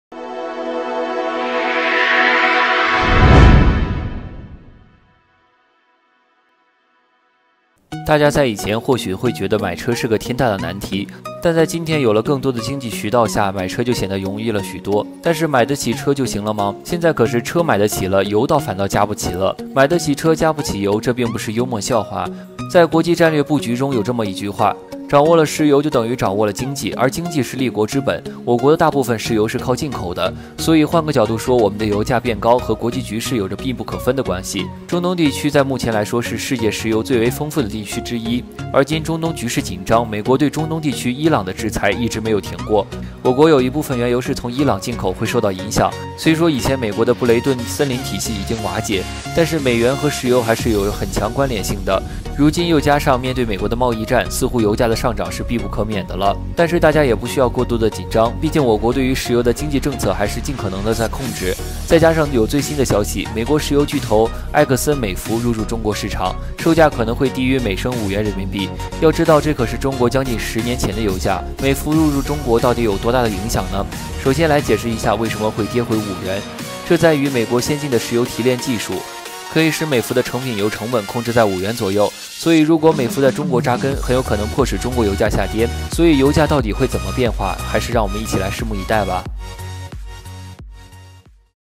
油價大地震？美國石油巨頭入駐中國，油價可能會跌回5元
大家在以前或許會覺得買車是個天大的難題，但在今天有了更多的經濟渠道下買車顯得容易了許多，但是買的起車就行了嗎？現在可是車買的起了，油反倒加不起了！“買的起車，加不起油”這並不是幽默笑話，而是存在於很多車主間共同的尷尬幽默，當今油價的飆升可謂“勢不可擋”，雖然間中也有下調過幾次，但是下調幅度小的可憐，總的來說現在高昂的油價成了很多車主的一大難關，汽車逐漸從交通工具變成了停車場的“收藏品”。